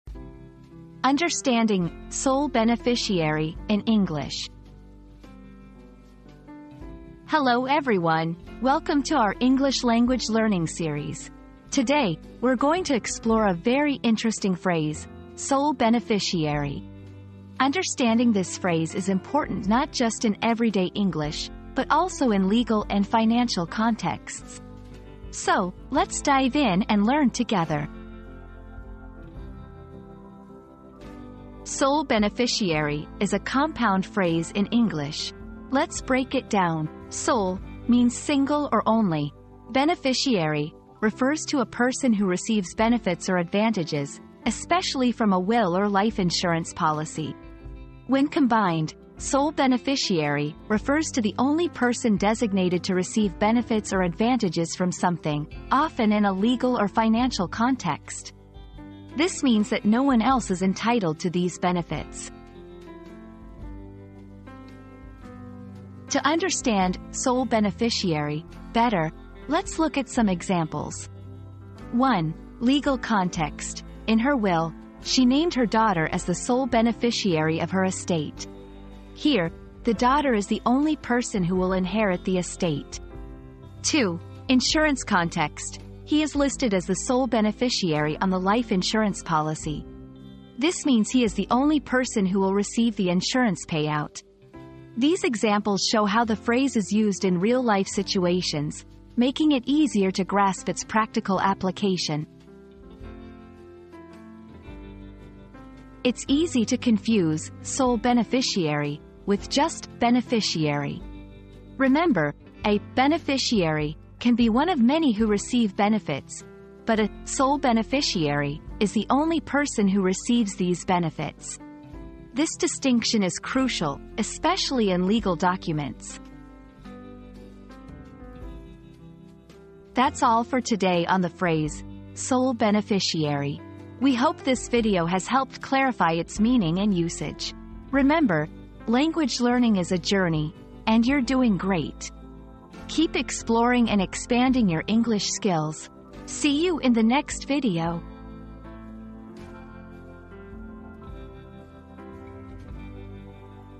Understanding "Sole Beneficiary" in English
Unlocking the Mystery: Decoding 'Sole Beneficiary' in English • Discover the secrets behind the enigmatic term 'Sole Beneficiary' in English law and gain a comprehensive understanding of its implications. Join us on this captivating journey to unravel the complexities and shed light on this intriguing legal concept.

00:00 • Introduction - Understanding "Sole Beneficiary" in English
00:31 • What Does "Sole Beneficiary" Mean?
01:05 • Examples in Context
01:45 • Common Misunderstandings